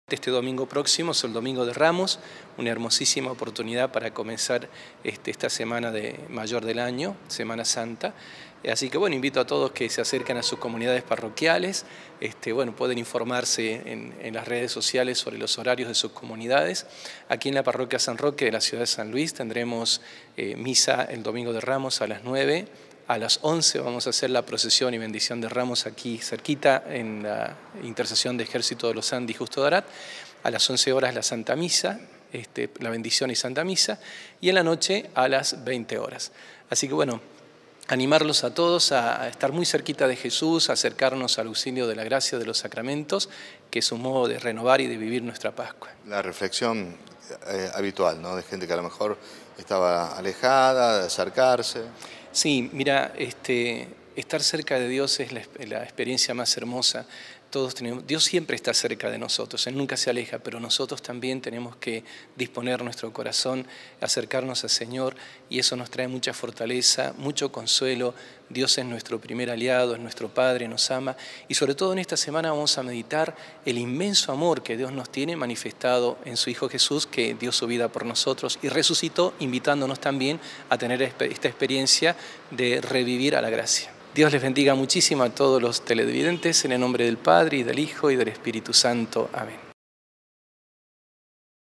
Inicia Semana Santa.
🔴 La comunidad católica celebrará este fin de semana la misa de Domingo de Ramos. Las parroquias preparan la jornada en la cual se realiza la tradicional bendición, por la entrada de Jesús en Jerusalem.